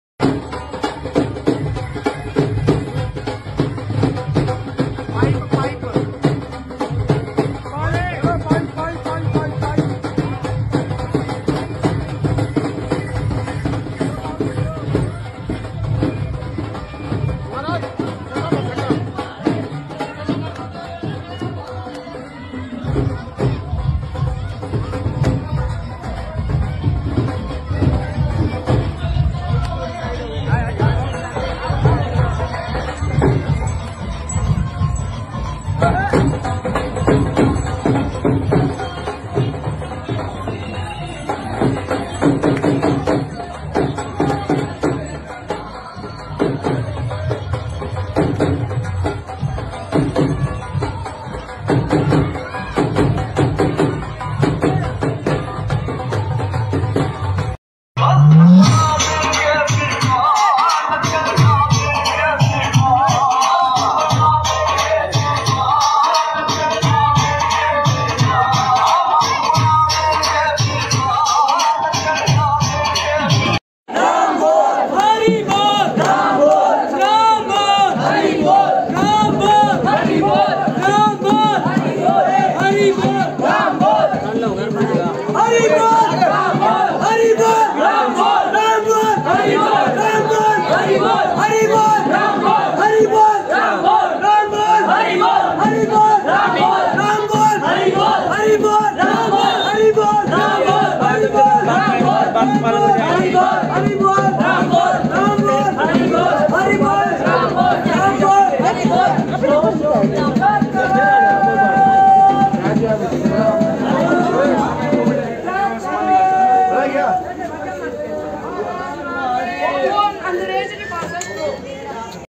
16•04•2022 | KARNAL SHOBHAYATRA | HANUMAN JANMOTSAV 2022 | HANUMAN JI LOVER
A tradition wherein the people have 41 Days fasts(Chaliya) or 21 Days fasts (Milk & Fruit only) with vow of celibacy(Bhramchar),lives outside their homes at sabha place in Temples.
They wear the attire of Hanuman called "Hanuman Swaroop "and roam about in streets, bestowing blessings on passers by and their devotees wishing them luck.
Residents invite these Hanuman ji  to their houses and organise ceremonies in which neighbours are invited to receive blessings from Hanuman Ji Swaroop.
These Hanuman Swaroops starts to wander 2 or 3 days before dusshera i.e from Durga Ashtmi till Vijay Dashmi.
After Dusshera ,they headover to Haridwar to get blessings from maa ganga to forgive for their mistakes and thank Hanumanji for all joy & their blessings.

Panipat Hanuman | Panipat Hanumanji |Panipat Dusshera|Panipat Hanuman Murti| Panipat Hanuman Sabha

I SHREE RAM🙏🙏
HANUMAN JI SWAROP DANCE